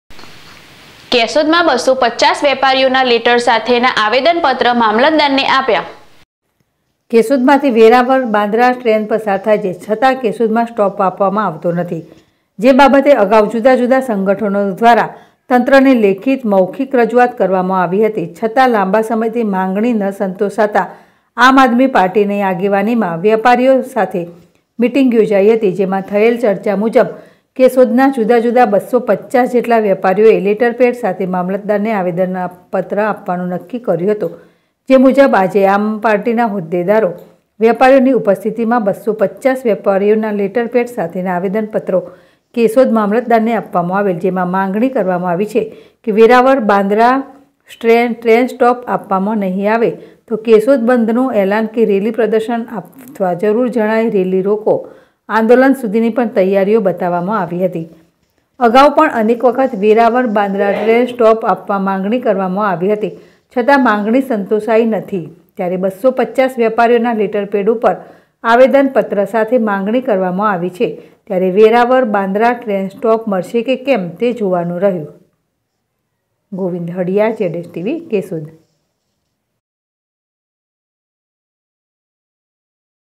કેશોદ માં ૨૫૦ વેપારીઓના લેટર સાથેના આવેદનપત્ર મામલતદારને આપ્યા
કેશોદ માં ૨૫૦ વેપારીઓના લેટર સાથેના આવેદનપત્ર મામલતદારને આપ્યા//ZSTVNEWS//